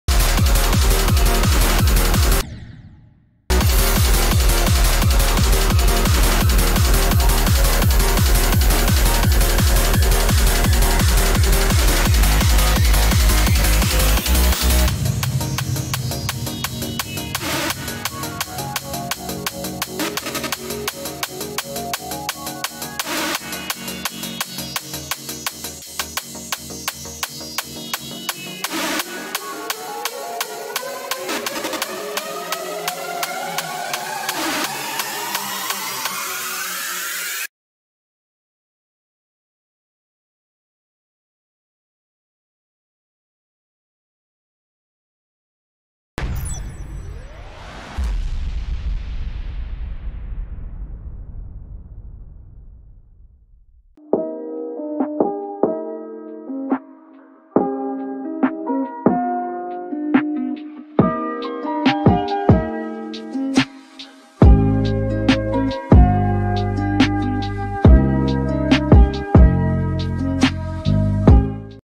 Diligence 67-100 huhu yo
This level is way harder than envy. So, I'd say this is about 110 or 115 on the list if it ever gets on there!